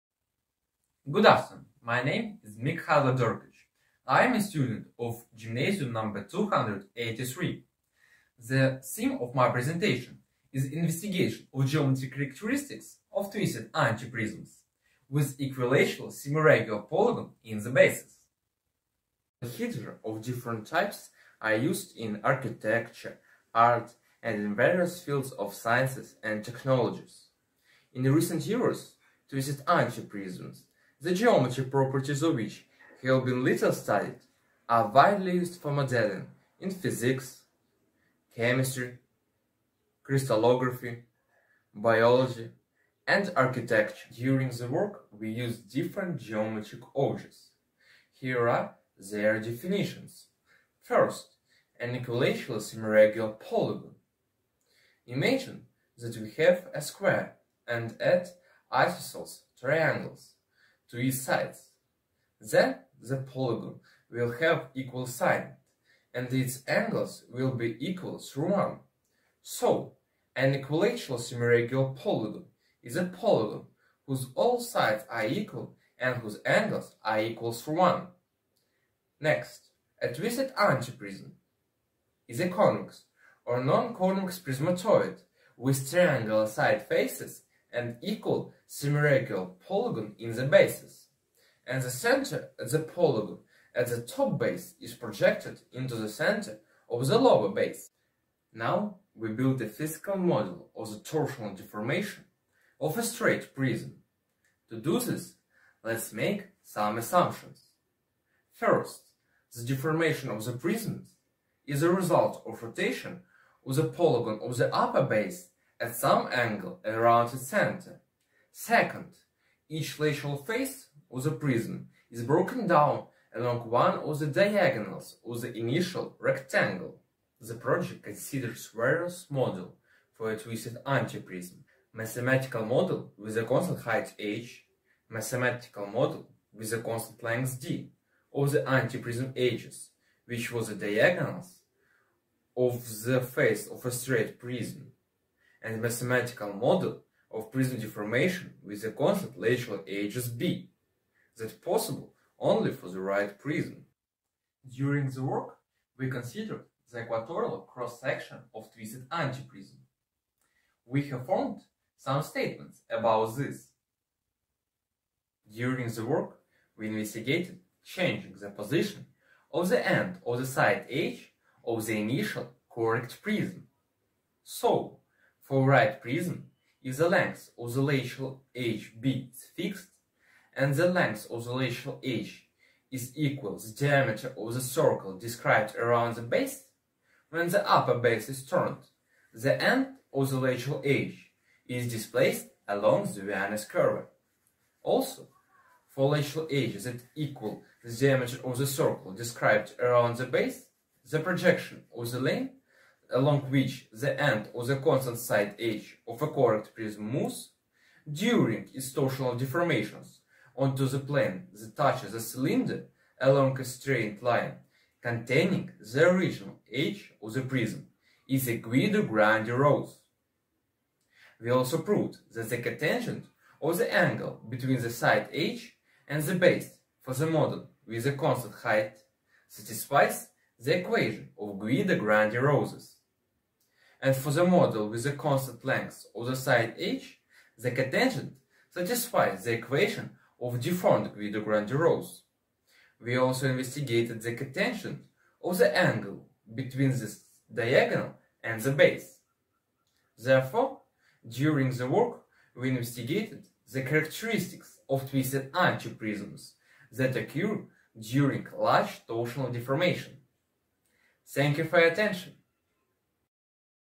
WISPO2021_UA-SM-HG-21082_INVESTIGATION OF GEOMETRIC CHARACTERISTICS
World Innovative Science Project Olympiad (WISPO) 2021 by Indonesia Scientific Society

Name: Mykhailo Derkach
School/University: Gymnasium №283, Kyiv Junior Academy of Sciences
Country: Ukraine
CODE: UA-SM-HG-21082
Category: Science and Mathematics

Share to your friend to get more Likes and Views
Curated by: Indonesia Scientific Society